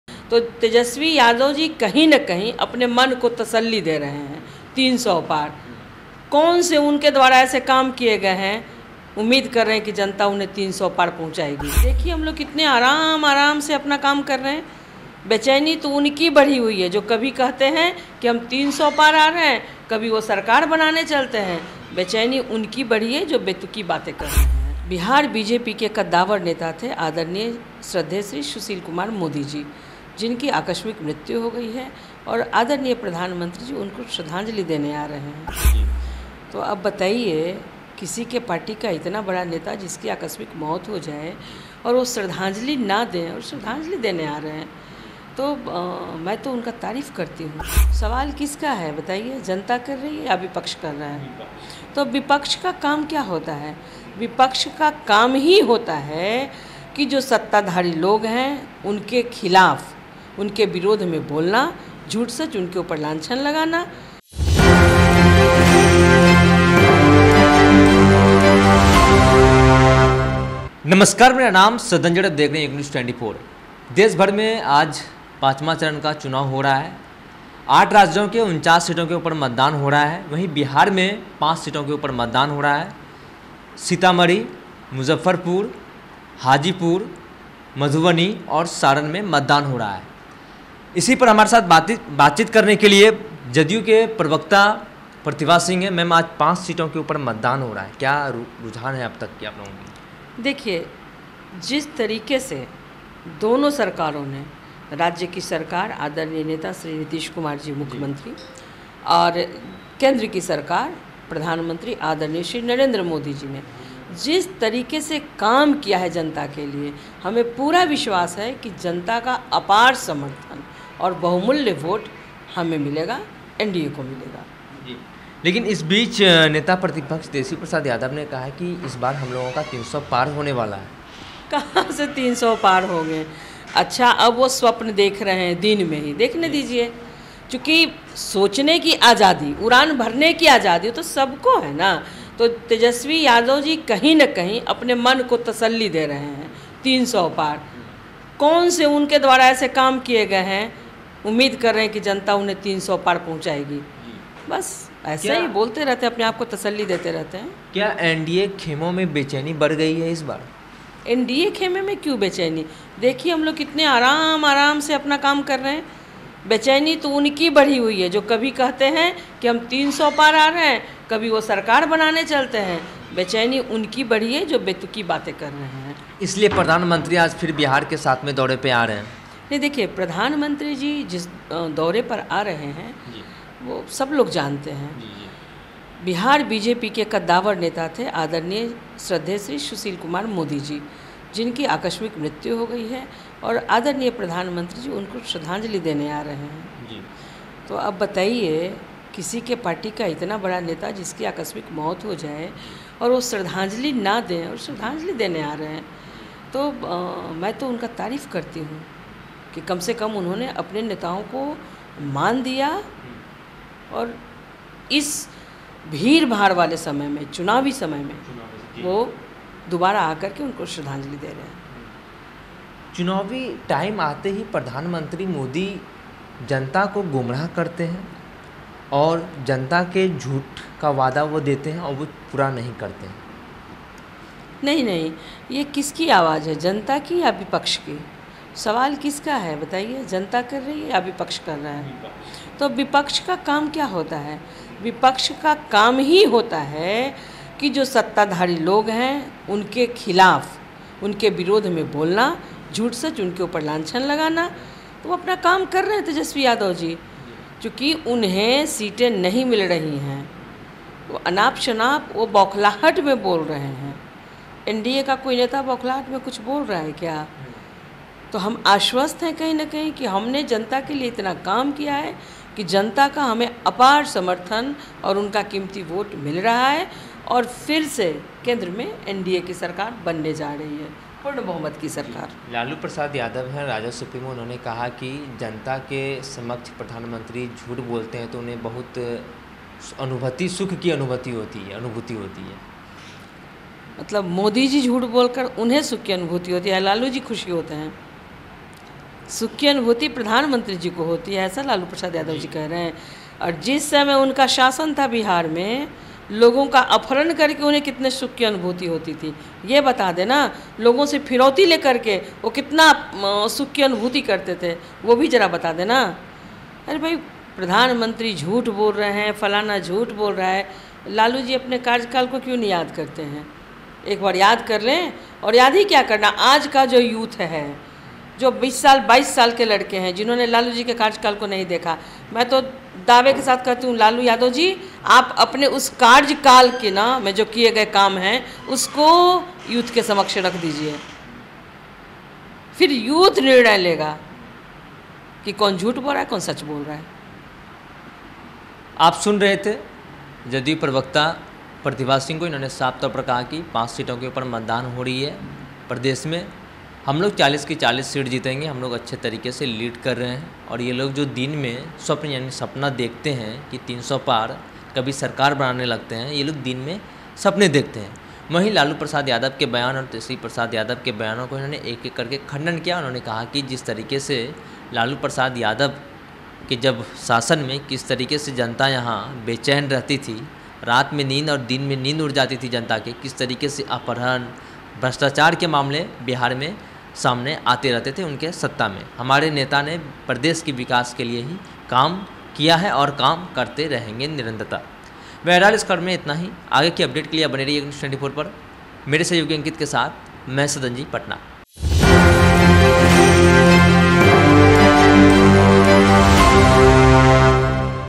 JDU प्रवक्ता Pratibha Singh ने Tejashwi पर किया हमला ! मन को तसल्ली दे रहे हैं, 300 पार सच्चाई का
/ eknews24

 / eknews24

 / ek_news_24